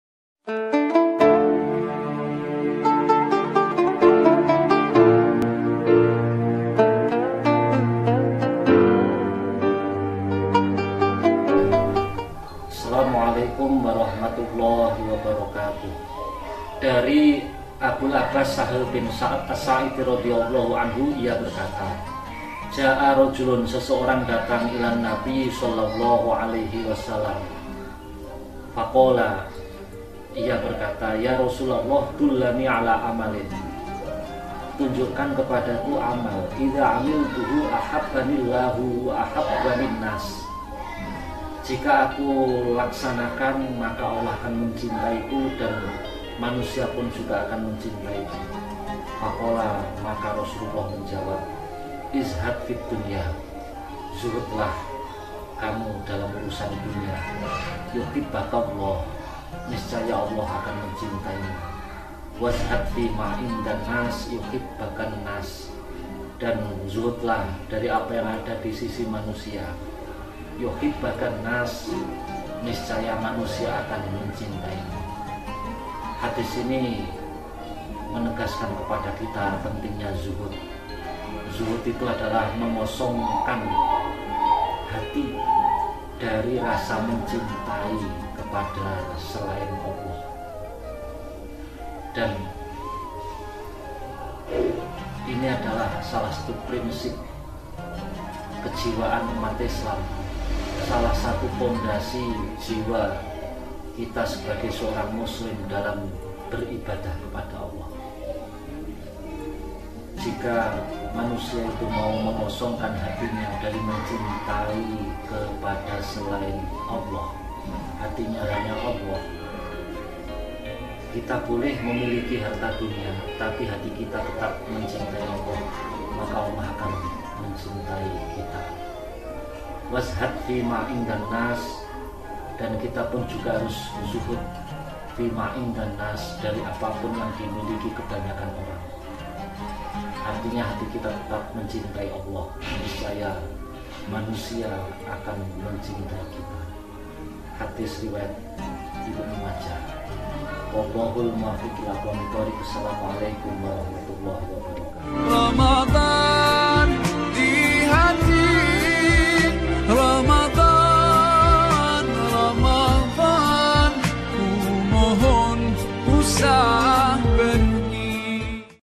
LDNU Mutiara Ramadlan #10 ASWAJA NU Center : Amalan yang Dicintai Allah dan Manusia
Mutiara Ramadlan #10
Aswaja NU Center Kab Kediri
Oleh: Ust. Dafid Fuadi, S.Ag
Amalan yang Dicintai Allah dan Manusia